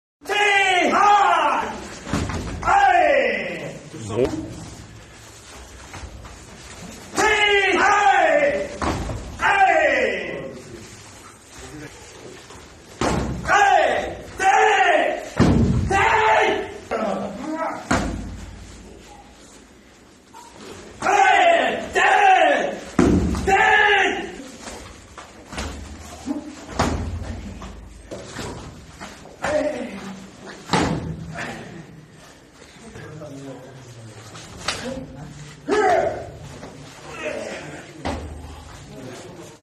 Jiin Bunkai
Kibadachi Shuto-uchi sotomawashi. Application of the sequence of Jiin.